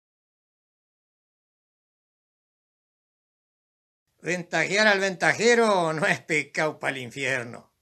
DICHOS 18
LOS DICHOS QUE NOS DECÍAN NUESTROS ABUELOS A MODO DE CONSEJO.